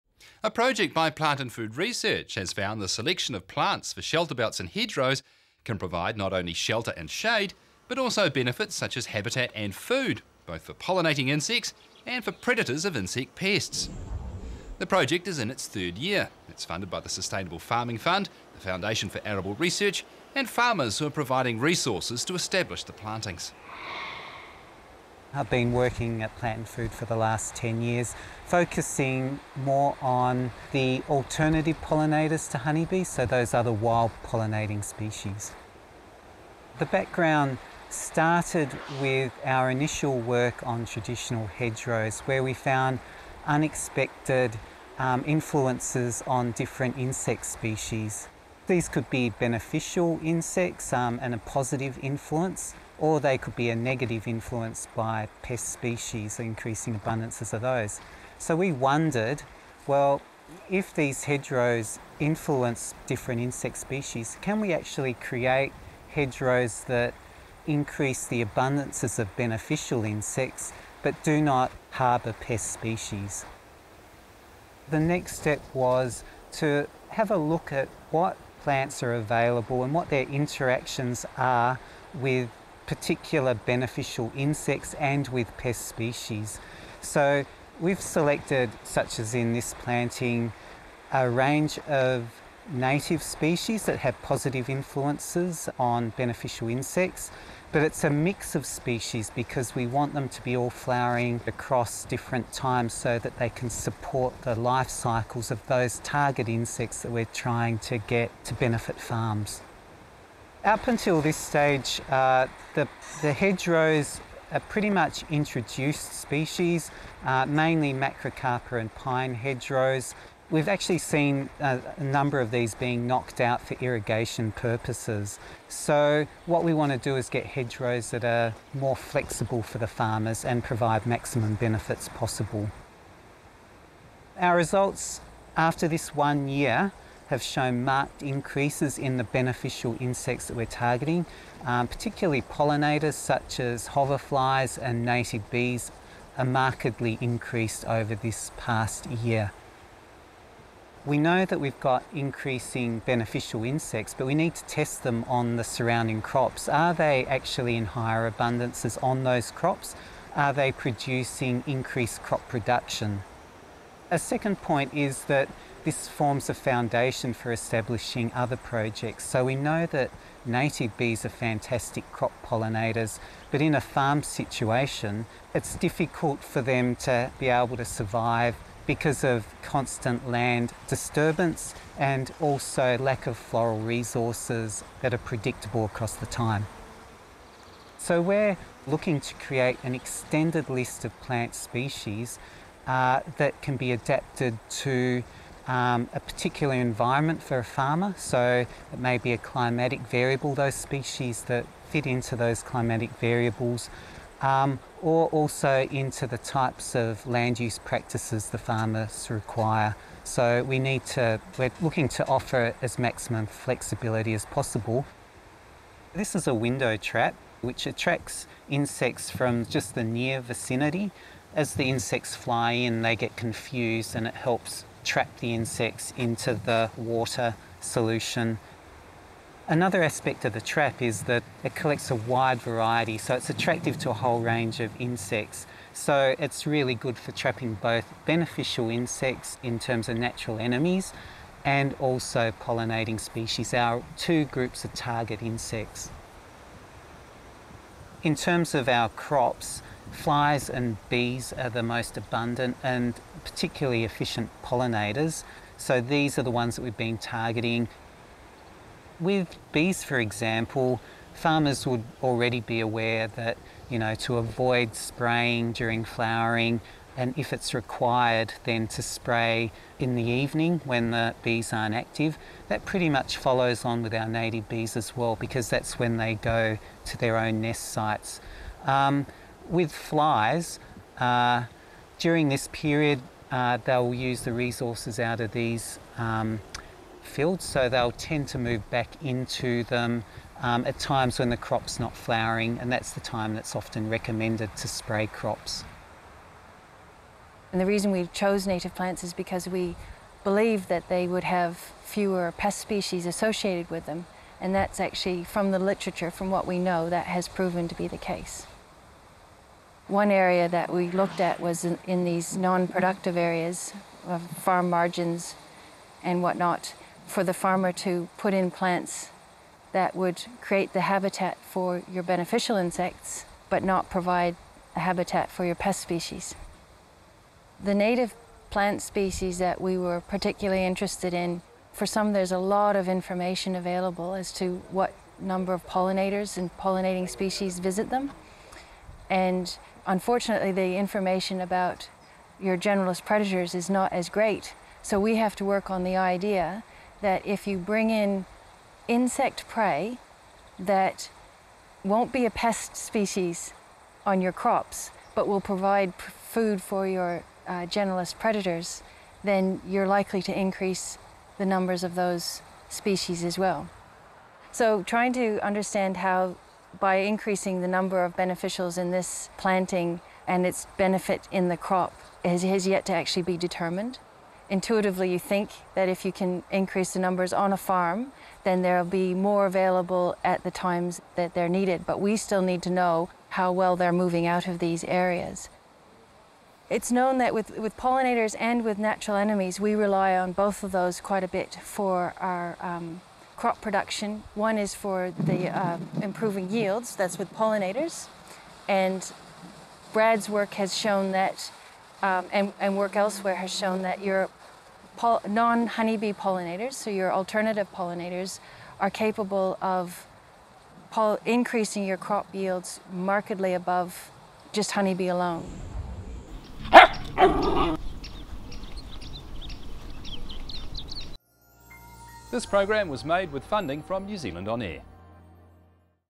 Diversity in Shelterbelts and Hedgerows
A Plant and Food Research, FAR, Sustainable Farming Fund project on plant diversity benefits